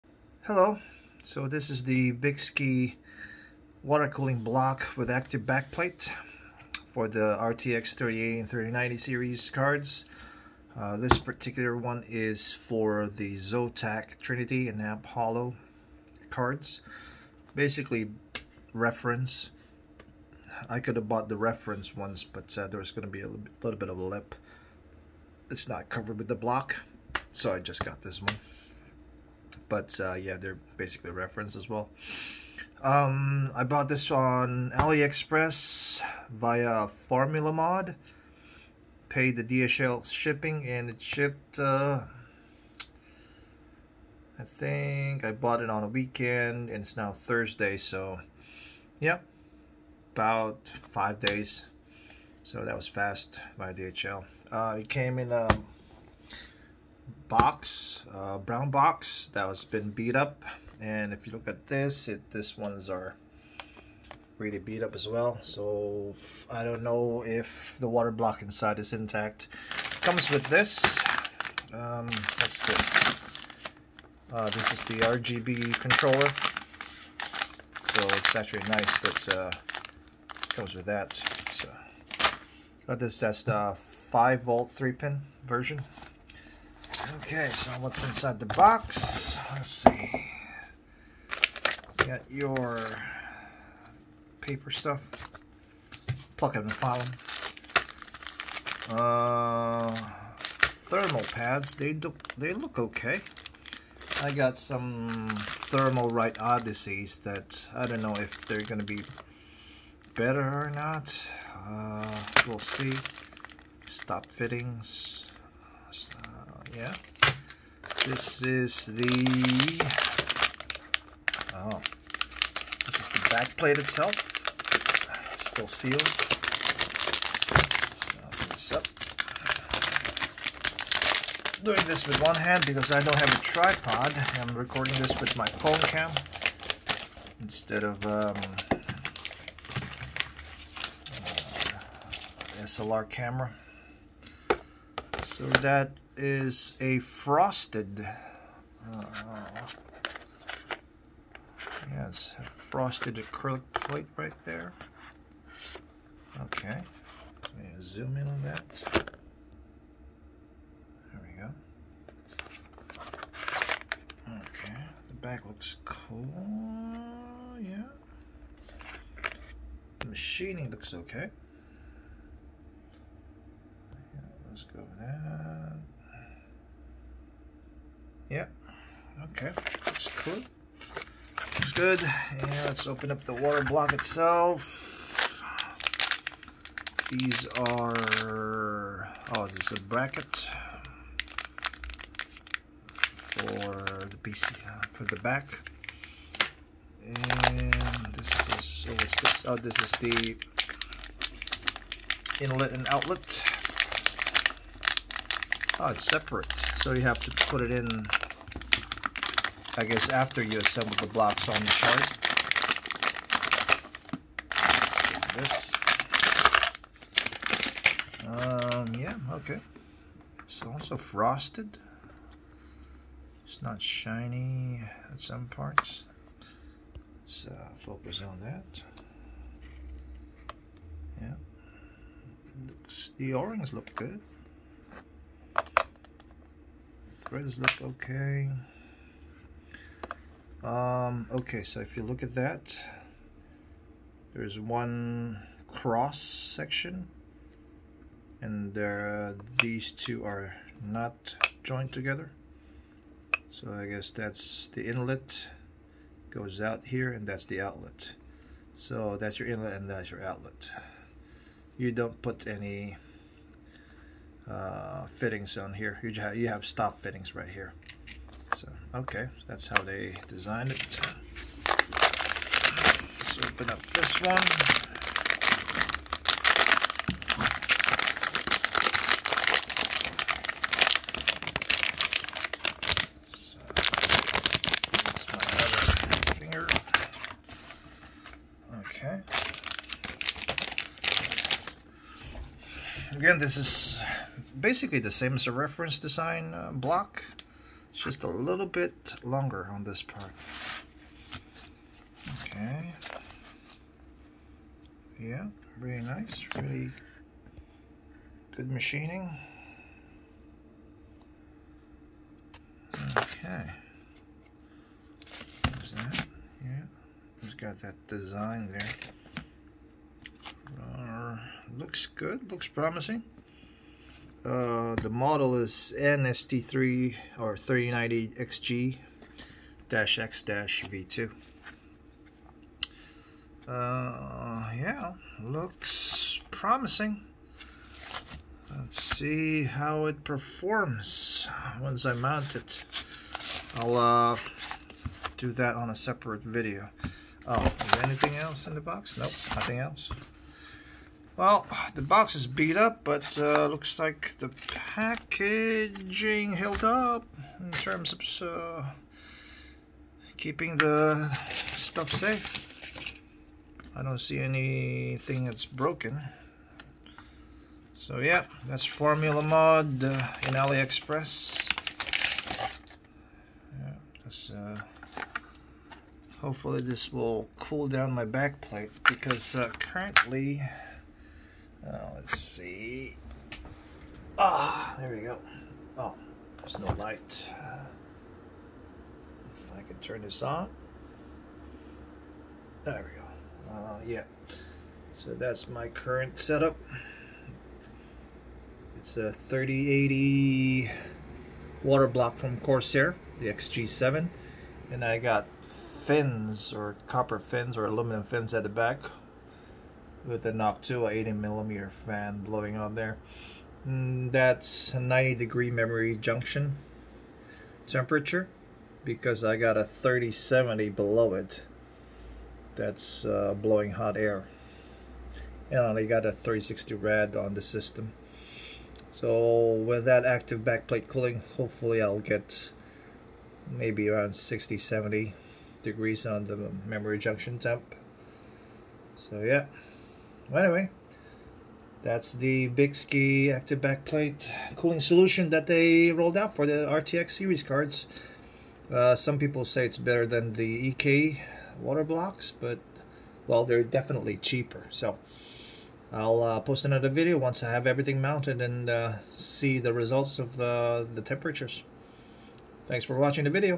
Unboxing Bykski RTX 3080/3090 waterblock with active backplate cooling
Bykski RTX 3080/3090 gpu block with active cooling backplate for the zotac 30xx cards trinity and amp holo series also fits reference cards
model: N-ST3090XG-X-V2